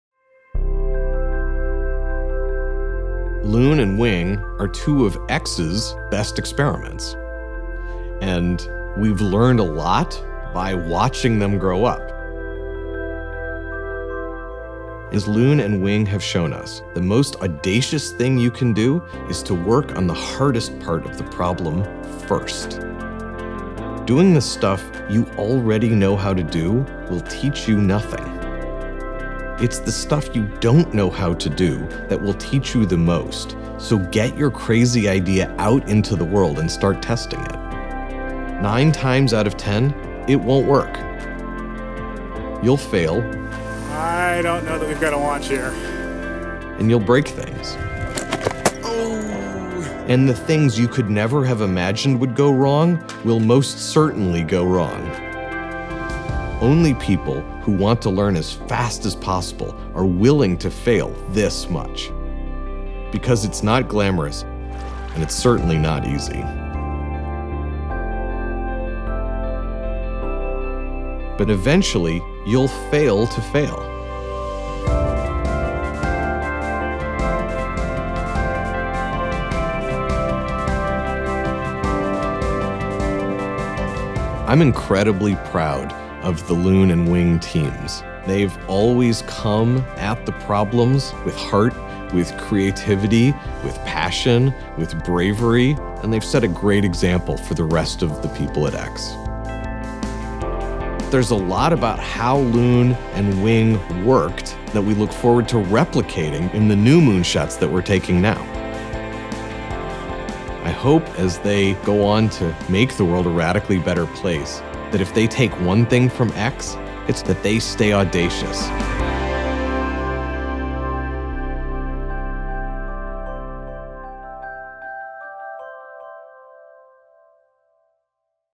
Loon and Wing: the story of two moonshots
Captain of Moonshots, Astro Teller, looks back at Loon and Wing's journeys while at X.